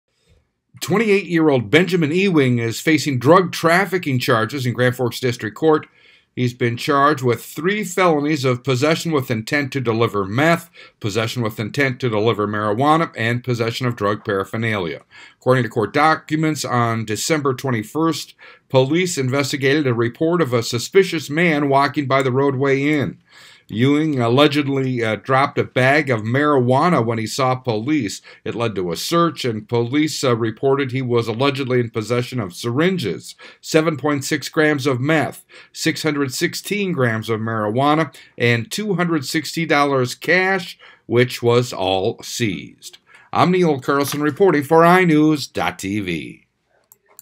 Man Facing Drug Trafficking Charges In Grand Forks
GRAND FORKS, ND (iNewZ.TV) Benjamin Ewing is facing drug trafficking charges in Grand Forks District Court.